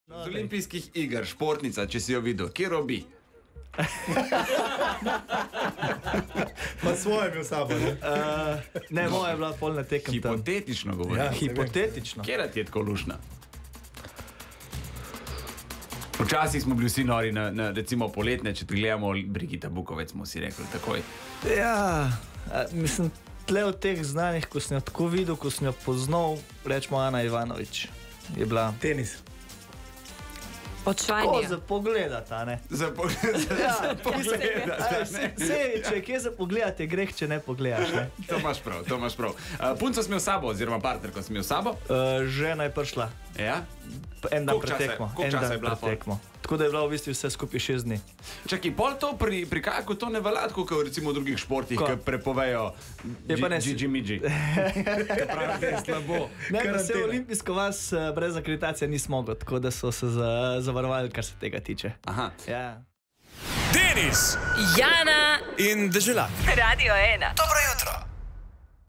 Peter Kauzer: "Ana Ivanović je za pogledat'!"
Peter Kauzer o tem, katera znana športnica mu je všeč.